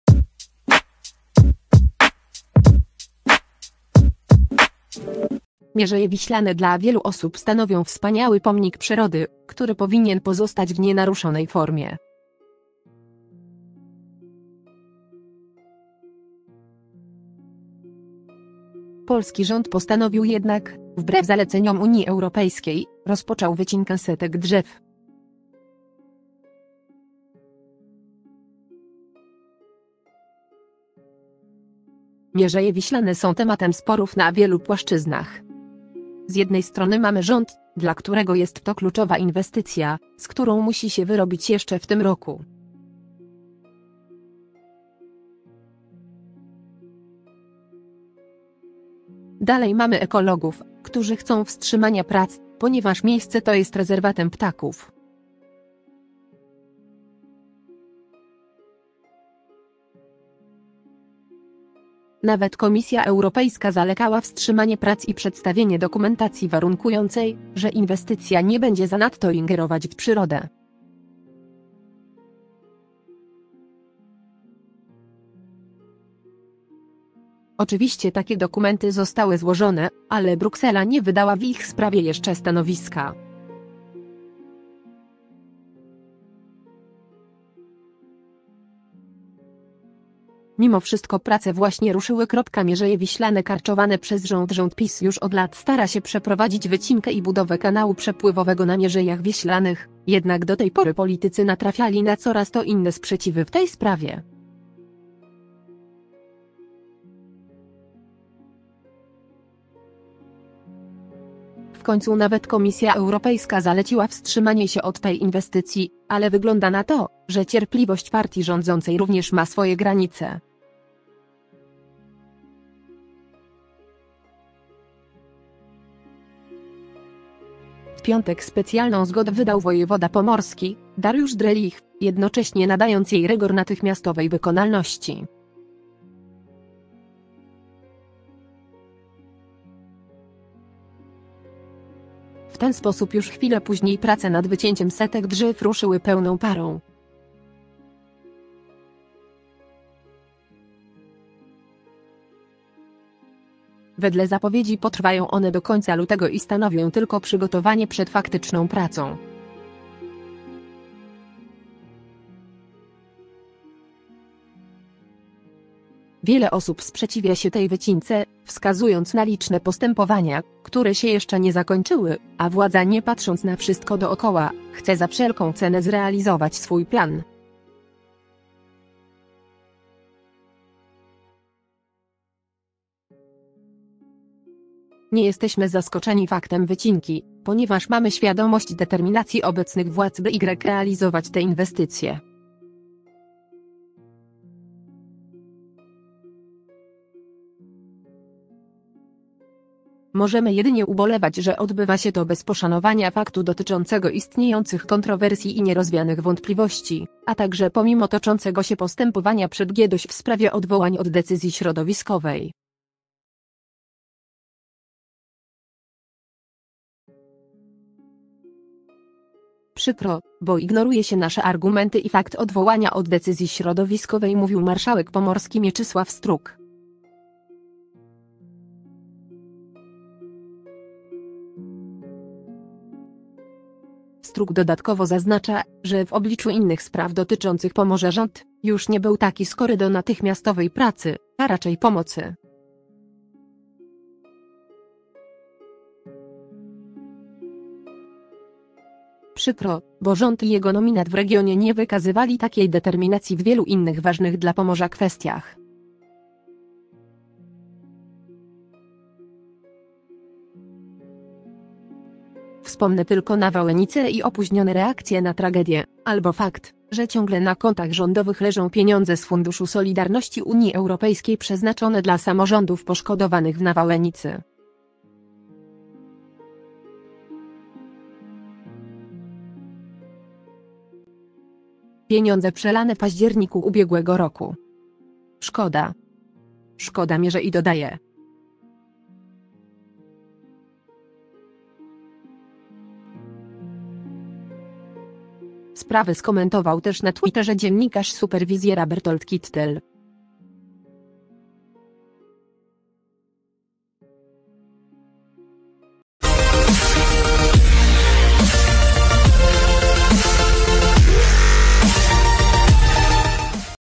Mamy zdjęcia! Trwa wycinka Mierzei wbrew zaleceniom Unii Europejskiej | Powiększenie
Dzięki za oglądanie! Jeśli lubisz wideo, zdobądź je i udostępnij znajomym. Kliknij tutaj, Mierzeje Wiślane dla wielu osób stanowią wspaniały pomnik przyrody, który powinien pozostać w nienaruszonej formie. Polski rząd postanowił jednak, wbrew zaleceniom Unii Europejskiej, rozpoczął wycinkę setek drzew. Mierzeje Wiślane są tematem sporów na wielu płaszczyznach. Z jednej strony mamy rząd, dla którego jest to kluczowa inwestycja, z którą musi się wyrobić jeszcze w tym roku. Dalej mamy ekologów, którzy chcą wstrzymania prac, ponieważ miejsce to jest rezerwatem ptaków. Nawet Komisja Europejska zalecała wstrzymanie prac i przedstawienie dokumentacji warunkującej, że inwestycja nie będzie zanadto ingerować w przyrodę. Oczywiście takie dokumenty zostały złożone, ale Bruksela nie wydała w ich sprawie jeszcze stanowiska. Mimo wszystko prace właśnie ruszyły.Mierzeje Wiślane karczowane przez rząd Rząd PiS już od lat stara się przeprowadzić wycinkę i budowę kanału przepływowego na Mierzejach Wiślanych, jednak do tej pory politycy natrafiali na coraz to inne sprzeciwy w tej sprawie. W końcu nawet Komisja Europejska zaleciła wstrzymanie się od tej inwestycji, ale wygląda na to, że cierpliwość partii rządzącej również ma swoje granice.    W piątek specjalną zgodę wydał wojewoda pomorski, Dariusz Drelich, jednocześnie nadając jej rygor natychmiastowej wykonalności. W ten sposób już chwilę później prace nad wycięciem setek drzew ruszyły pełną parą. Wedle zapowiedzi potrwają one do końca lutego i stanowią tylko przygotowanie przed faktyczną pracą. Wiele osób sprzeciwia się tej wycince, wskazując na liczne postępowania, które się jeszcze nie zakończyły, a władza nie patrząc na wszystko dookoła, chce za wszelką cenę zrealizować swój plan.   – Nie jesteśmy zaskoczeni faktem wycinki, ponieważ mamy świadomość determinacji obecnych władz by realizować tę inwestycję. Możemy jedynie ubolewać że odbywa się to bez poszanowania faktu dotyczącego istniejących kontrowersji i nierozwianych wątpliwości, a także pomimo toczącego się postępowania przed GDOŚ w sprawie odwołań od decyzji środowiskowej. Przykro, bo ignoruje się nasze argumenty i fakt odwołania od decyzji środowiskowej – mówił marszałek pomorski Mieczysław Struk. Struk dodatkowo zaznacza, że w obliczu innych spraw dotyczących pomorza rząd, już nie był taki skory do natychmiastowej pracy, a raczej pomocy. – Przykro, bo rząd i jego nominat w regionie nie wykazywali takiej determinacji w wielu innych ważnych dla Pomorza kwestiach. Wspomnę tylko nawałnicę i opóźnione reakcje na tragedię, albo fakt, że ciągle na kontach rządowych leżą pieniądze z Funduszu Solidarności Unii Europejskiej przeznaczone dla samorządów poszkodowanych w nawałnicy. Pieniądze przelane w październiku ubiegłego roku. Szkoda. Szkoda Mierzei – dodaje. Sprawę skomentował też na Twitterze dziennikarz Superwizjera Bertold Kittel.